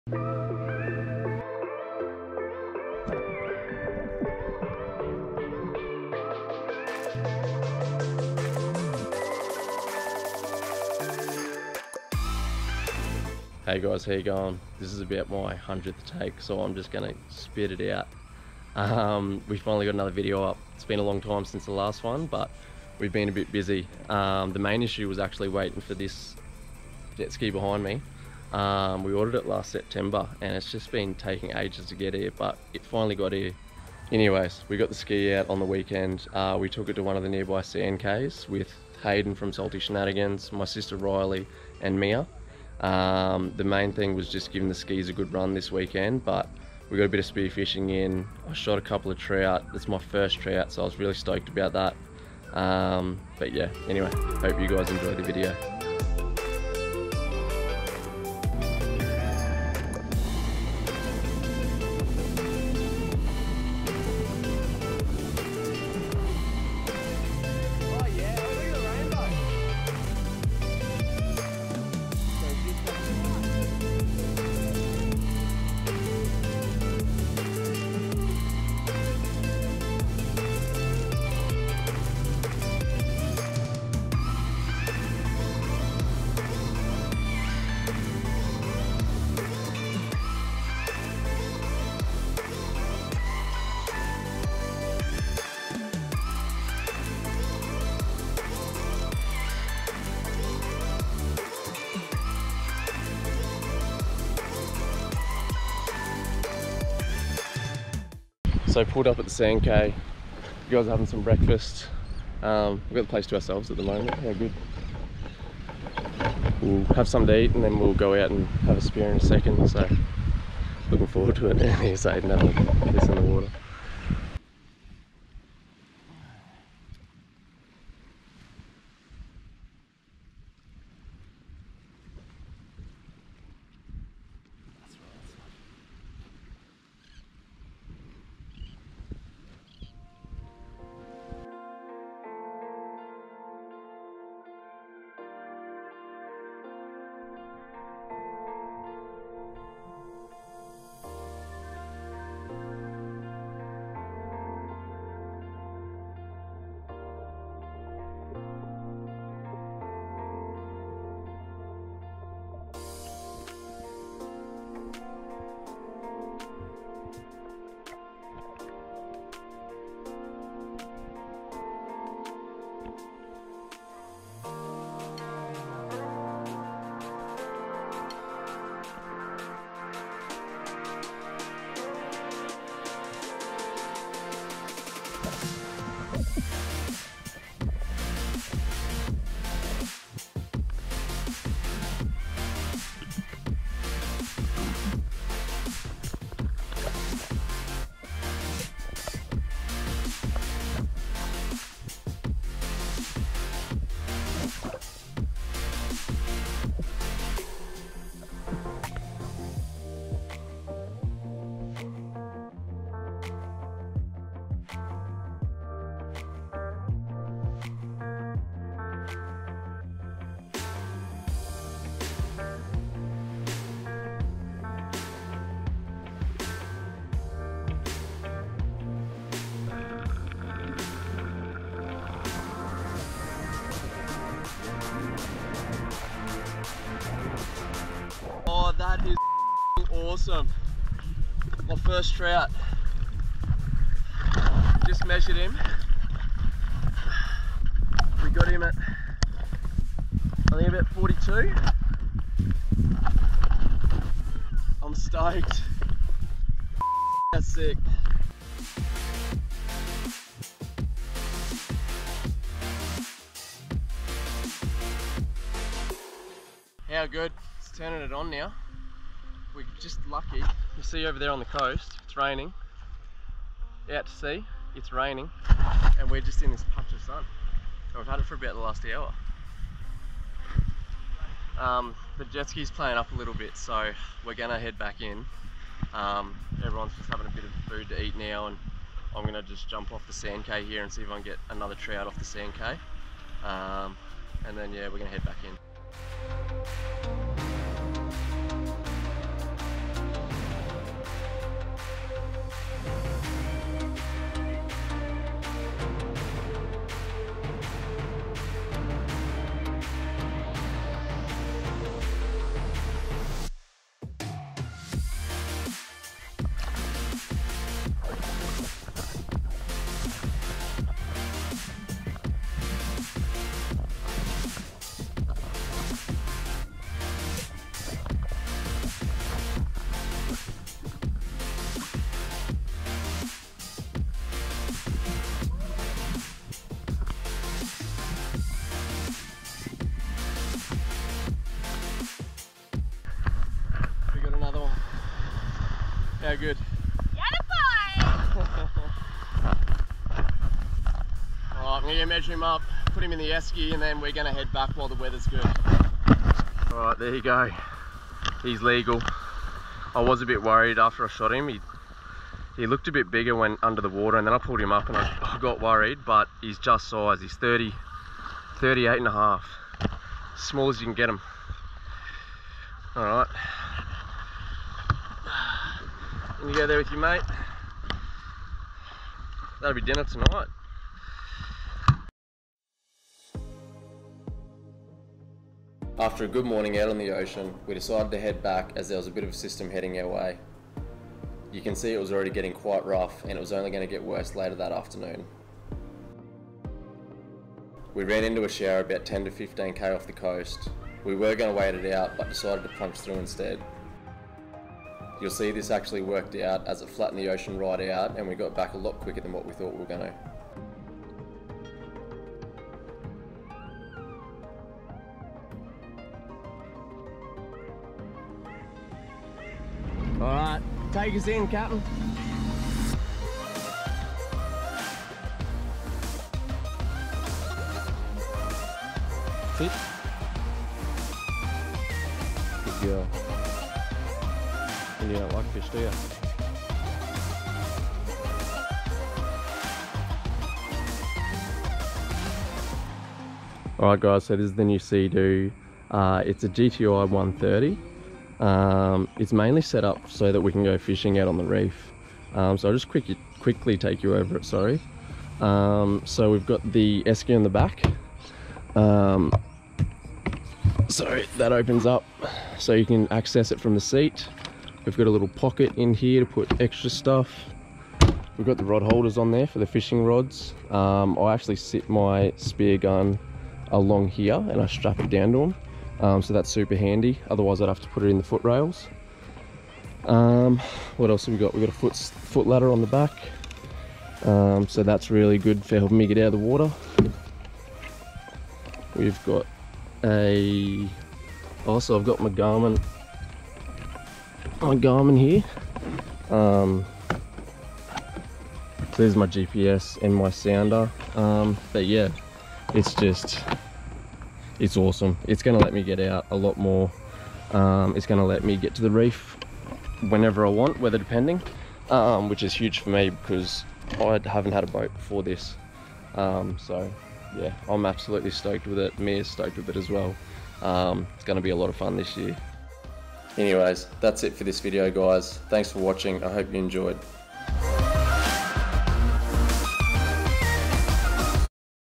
SPEARFISHING THE GREAT BARRIER REEF | Shooting my first coral trout
We head out to a nearby reef and park up on a sand cay for the day! Spearfishing, snorkelling and some amazing scenery to go along with it. I shoot my first ever coral trout which only has me even more hooked on spearfishing.

Instagram is @oceanantics.au